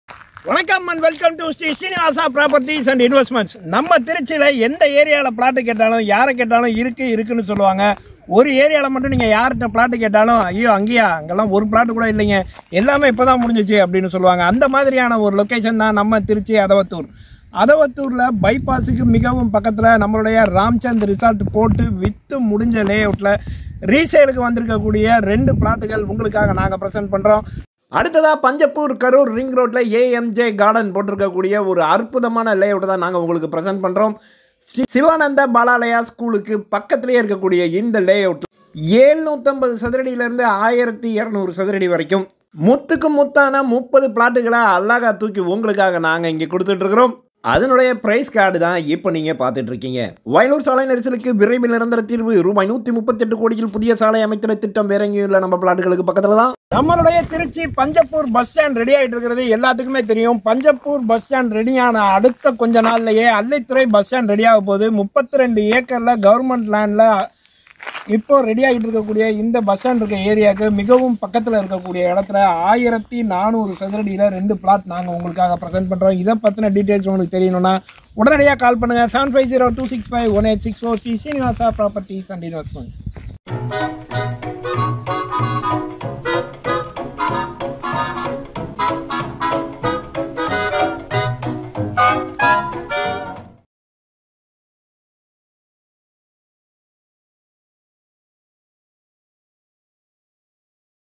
வயலூர் அருகில் வீட்டு மனைகள் விற்பனைக்கு
மெட்ரோ ரயில் நிலையம் மற்றும் பஞ்சப்பூர் கரூர் தேசிய நெடுஞ்சாலை மிக அருகில் பலதரப்பட்ட வீட்டு மனைகள் விற்பனைக்கு

தொடர்பிற்கு
Sri Srinivasaa Properties and Investments
No 114, Dhamodharan Krishnan Kovil Street, Keezhavaasal, Srirangam, Tiruchirappalli 620006